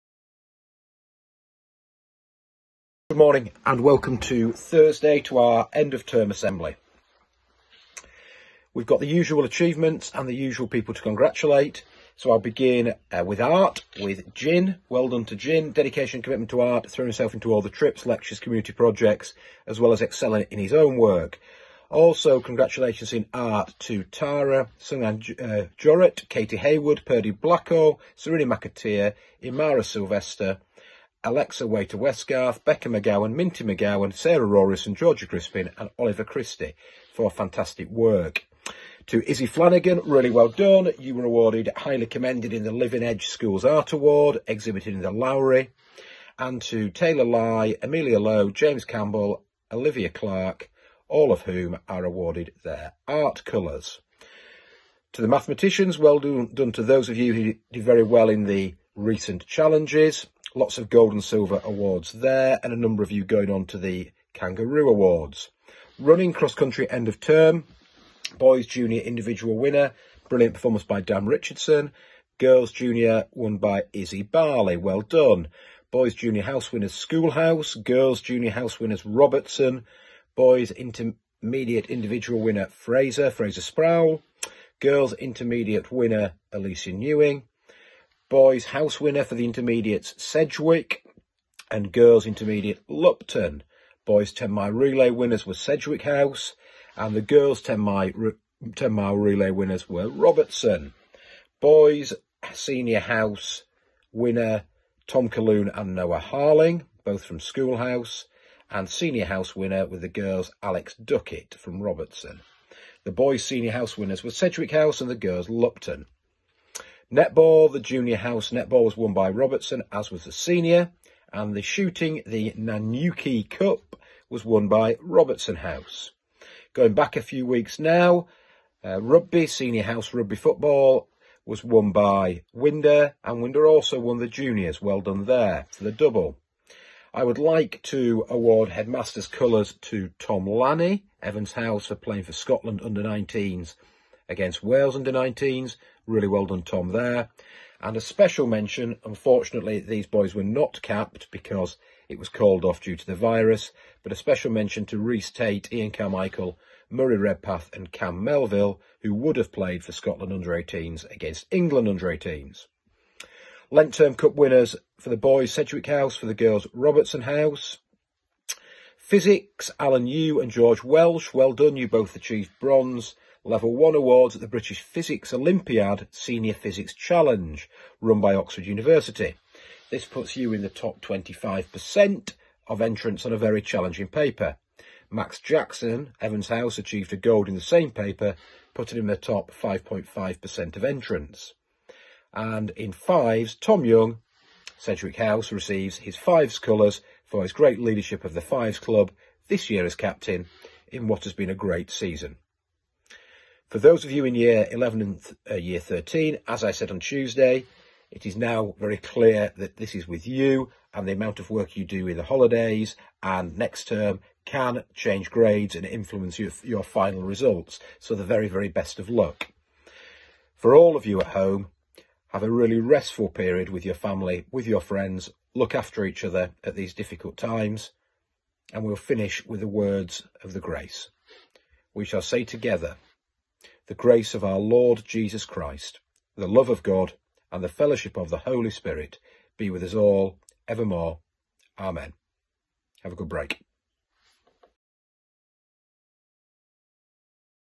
Sedbergh School Virtual Assembly - 26 March 2020
The fourth, and final, virtual assembly of week, delivered by Headmaster Mr Harrison.